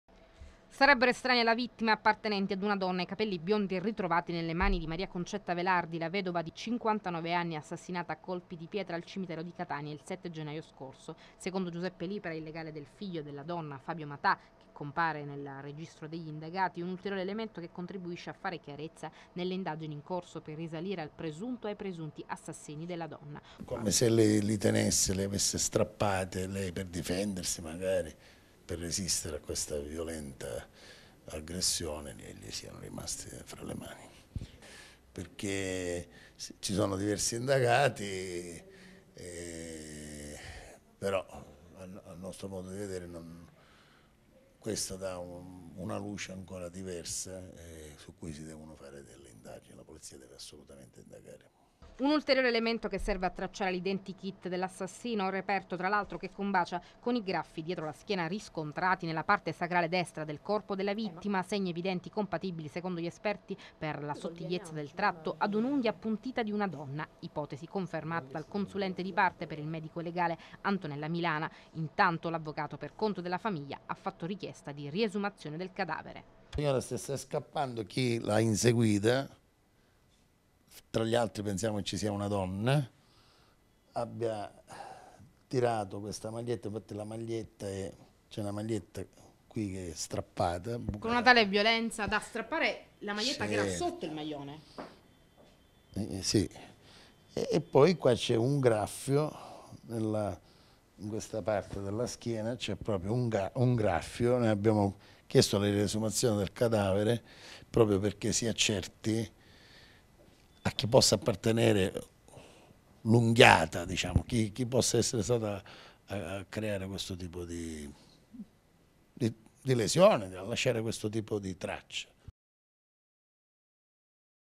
Capelli di donna nella mani di Maria Concetta Velardi - Serivizio Rei Tv
Capelli di donna trovati nella mani di Maria Concetta Velardi, la donna assassinata al cimitero di Catania lo scorso 7 gennaio. Si rafforza l'ipotesi del coinvolgimento di una donna nell'omicidio.